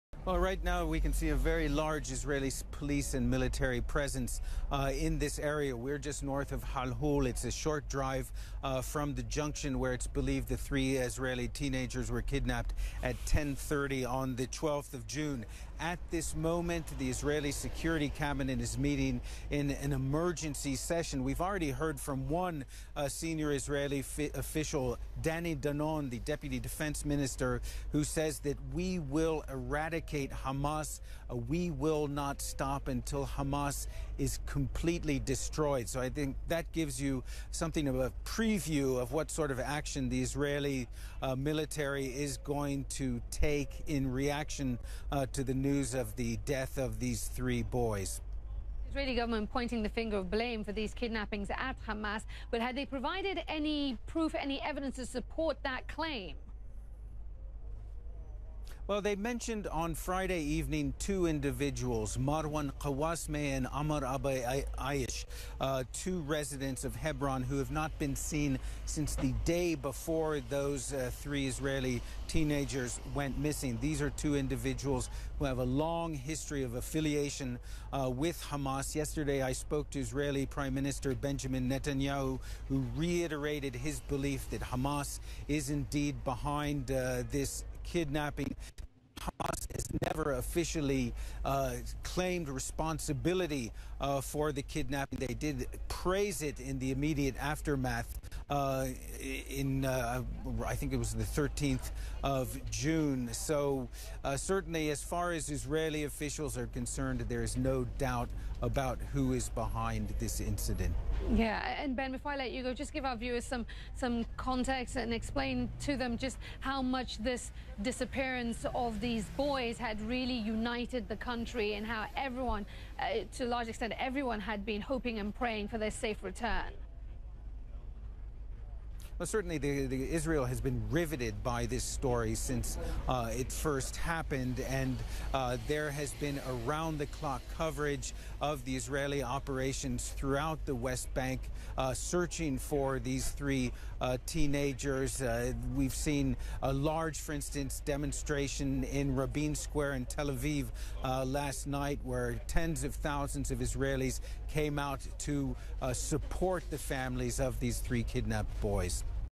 Israeli Deputy Defense Minister vows Israel "will completely eradicate" Hamas
Israel's Deputy Minister of Defense Danny Danon reacted to the discovery of the bodies of Naftali Frankel, Gil-Ad Shaer, and Eyal Yifrach today.
"I was filled with deep sadness upon hearing of the murders of Naftali Frankel, Gil-Ad Shaer, and Eyal Yifrach by Hamas terrorists.  All of Israel mourns with these brave families who will forever hold a special place in the collective hearts of our nation.
In their memories, we must ensure that this tragic end be turned into an opportunity to create a better and safer Israel.  Israelis have the willingness and the fortitude necessary to endure the hardships of a long-lasting operation aimed at eradicating Hamas.
We will not stop until Hamas is completely defeated.  The homes of the terrorists must be demolished and their arms caches destroyed.  Our mission will not be complete until all the terror organizations are deterred from attacking Israelis and understand once and for all that the people of Israel will not be threatened
I call on the international community to end all aid to the Palestinian Authority and its Hamas-backed government.  President Abbas cannot continue to claim to want peace with Israel, while at the same time partnering with Hamas as they kidnap and brutally murder teenagers."